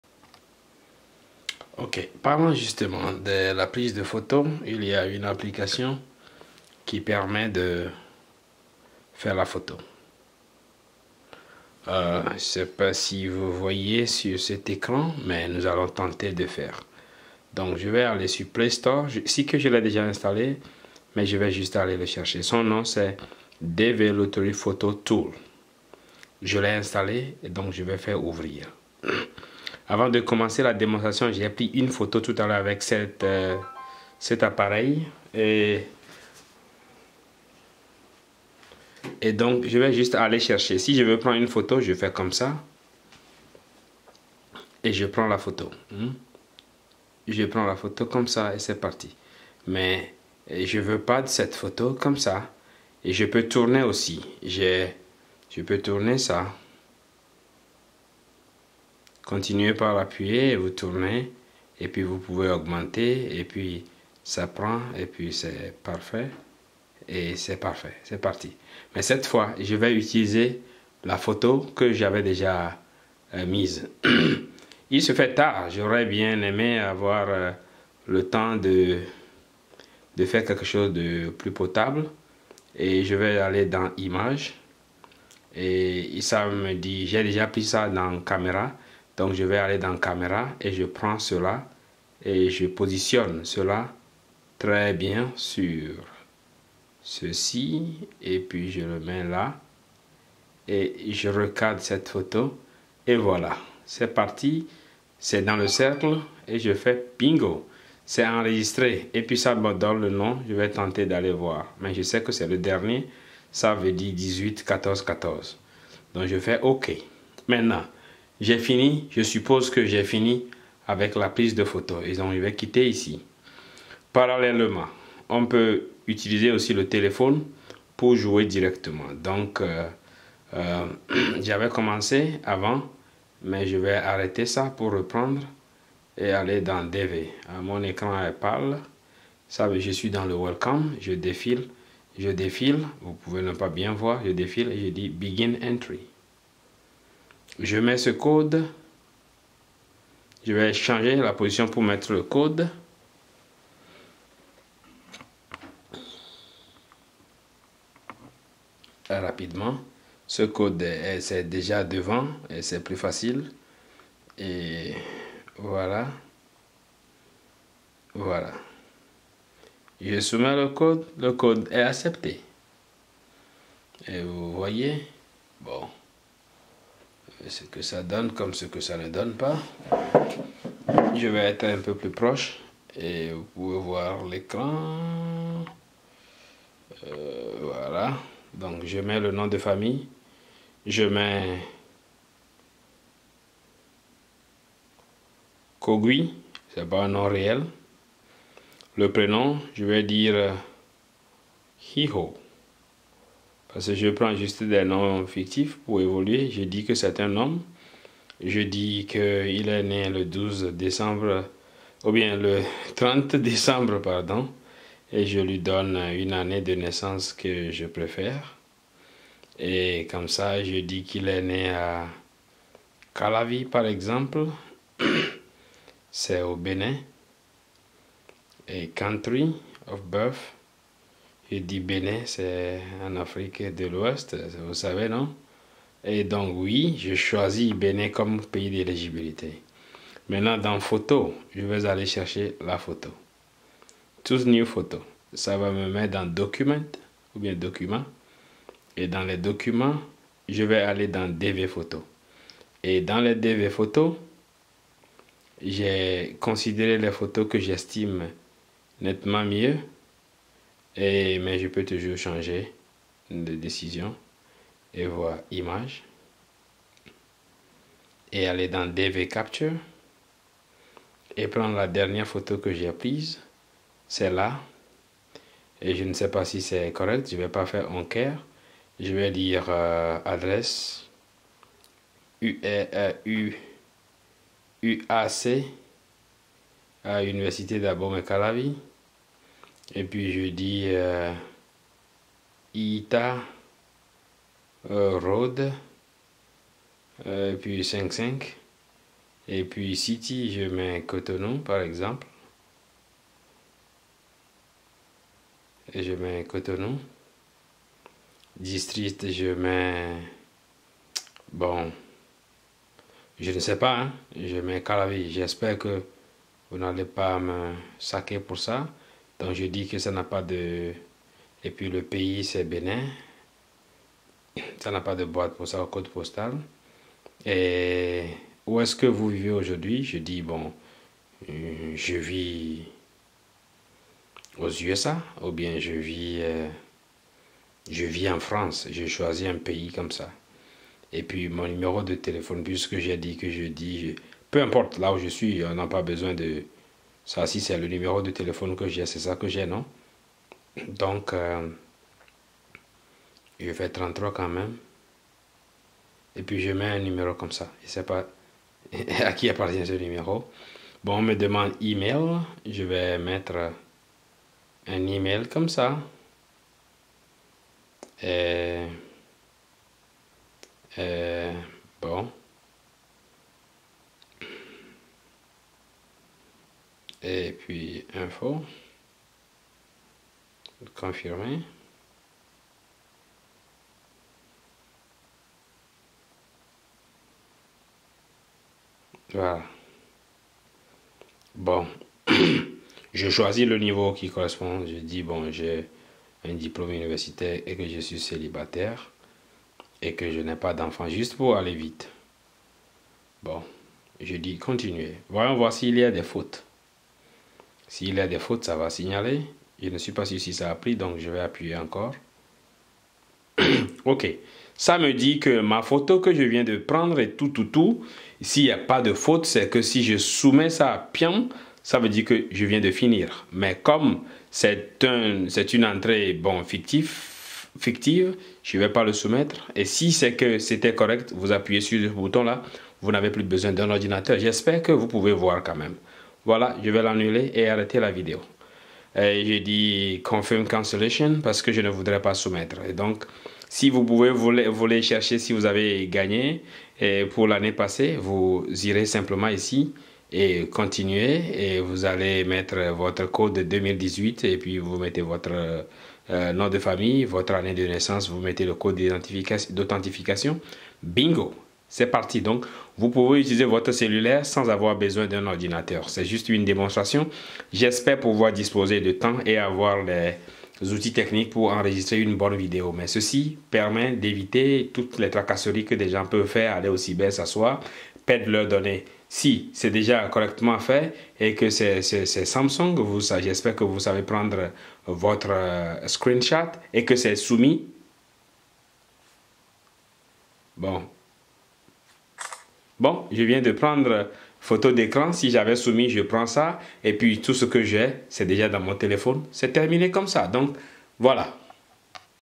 DV Lottery Photo Tool (outil de photo pour DV): application mobile
DV Lottery Photo Tool  est une application nouvellement développée et qui permet d'aller plus vite dans l'acquisition et le traitement de des photos répondant aux exigences du programme de diversité culturelle des USA ou DV Lottery VISA.
Merci au développeur Arjen de Klerk.
C'est bon pour les androids et iPhones.

❤️ 💜 Aimez 💛
💙 🔄 Partagez ➡️

Si vous avez des questions privées, contactez-moi sur ma page Facebook